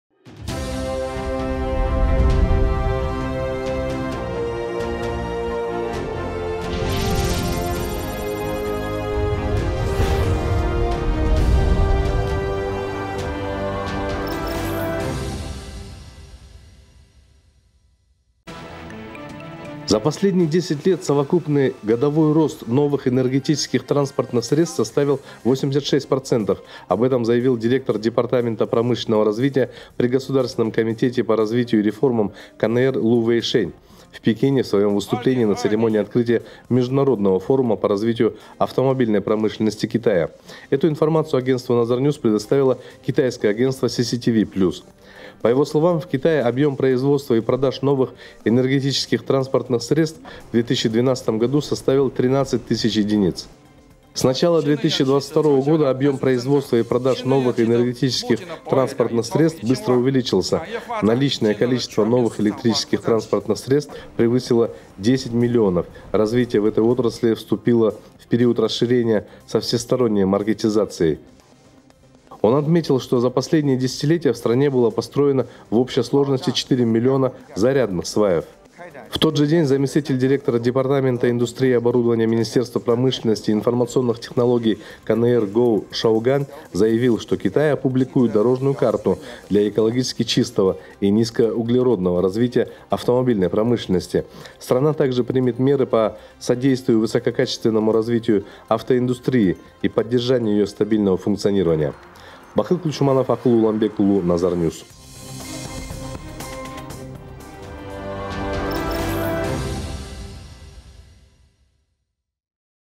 Китай вступил за развитие новых энергетических транспортных средств
За последние десять лет совокупный годовой рост новых энергетических транспортных средств составил 86%. Об этом заявил директор департамента промышленного развития при Государственном комитете по развитию и реформам КНР Лу Вэйшэн 3 сентября в Пекине в своем выступлении на церемонии открытия Международного форума по развитию автомобильной промышленности Китая. Эту информацию агентству «NazarNews» предоставило китайское агентство «CCTV-plus».